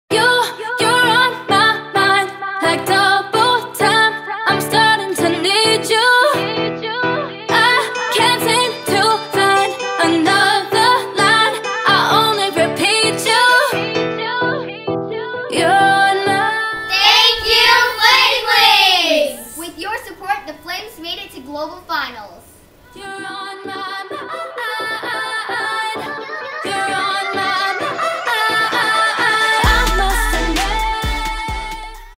Thank You from DI team!
The DI Team, The Flames, made a short video to thank the Eich students for helping them fundraise for Global Finals with Destination Imagination.